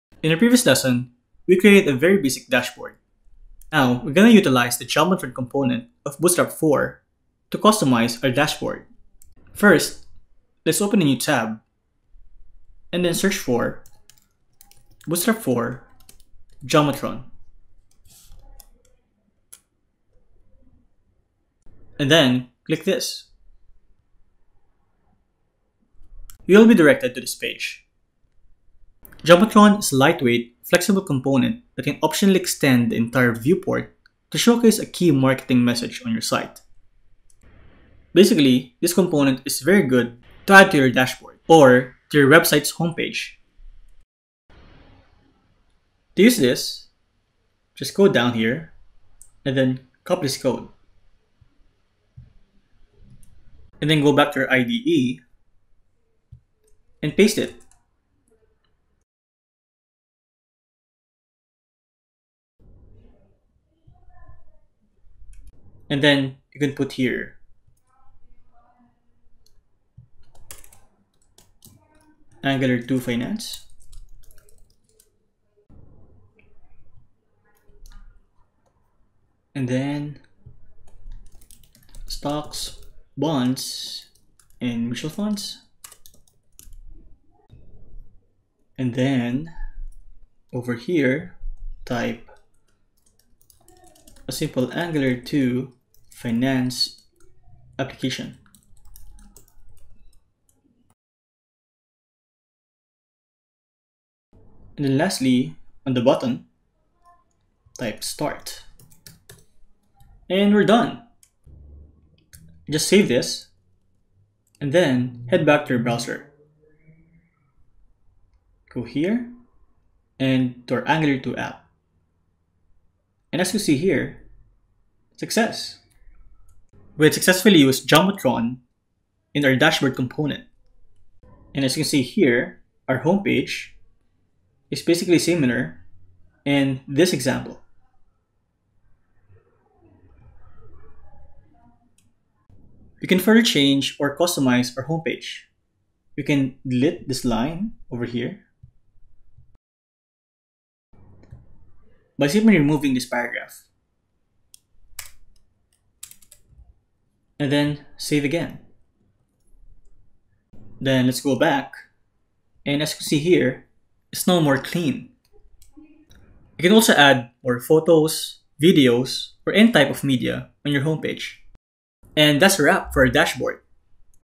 Bootstrap Lecture 7 - Customizing Your Dashboard with Jumbotron
In this lesson, we will learn how to customize our dashboard with the use of Jumbotron.